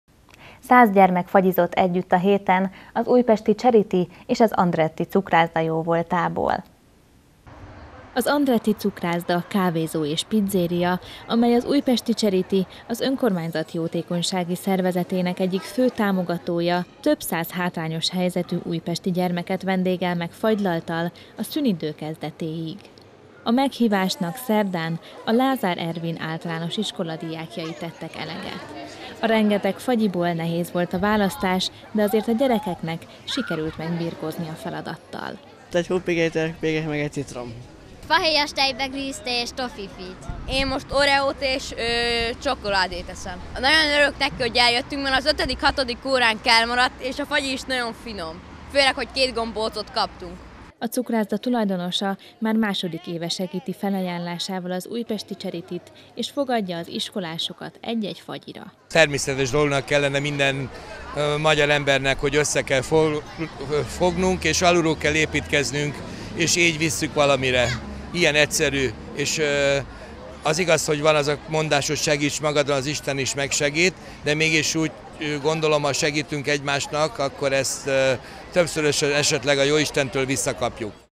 Száz gyermek fagyizhatott Újpesten
Száz gyermek fagyizott együtt a héten az Újpesti Cseriti és az Andretti Cukrászda jóvoltából.